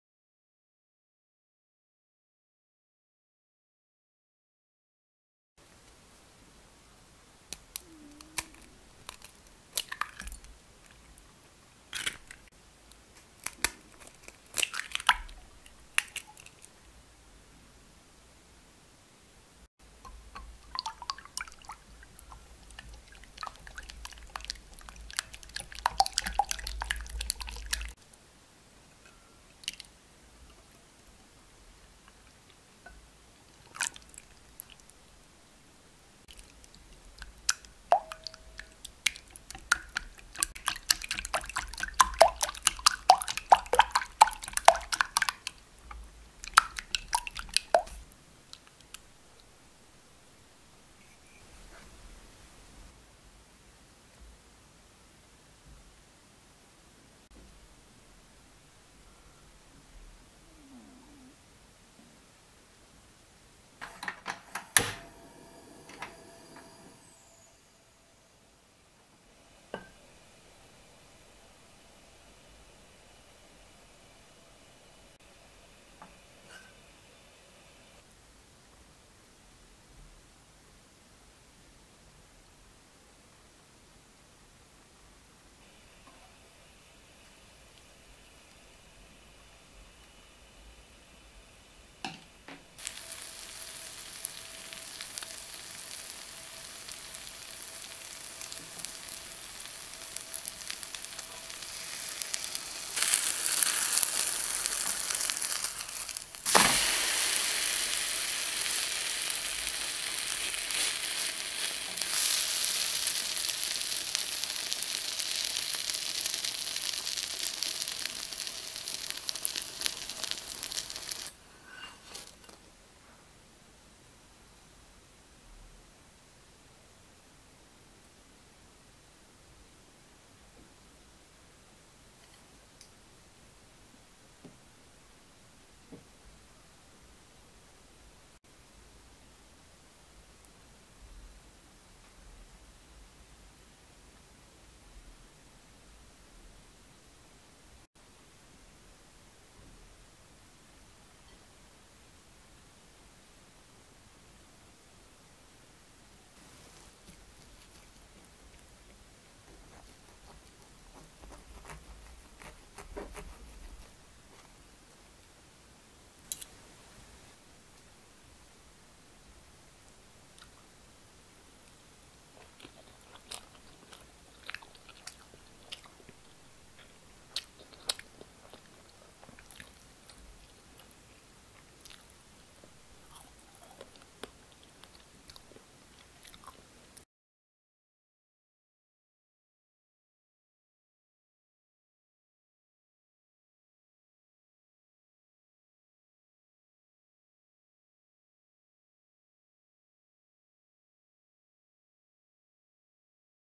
French toast 프렌치토스트 만들기フレンチトースト/ 베이킹 한세hanse
출출해서 셋이서 뭘 먹을까 하다가 .. 프렌치 토스트 하나씩 먹기로했어요
간단하고 맛도 괜찮아서 자주해먹어요 ㅎㅎ

시판식빵 약 4~5개 정도 적셔 만들수 있어요 :)

재료 ingredient

식빵 4 bread
계란 2 egg
우유 120ml milk
계피가루 2g cinnamon powder
꿀 honey
버터 butter

*다른과일과 곁들여도 좋고 그냥 드셔도 됩니다
시나몬가루는 제외해도 됩니다
꿀대신 연유 혹은 설탕을 드시면되구요
버터대신 식용유로 구워도됩니다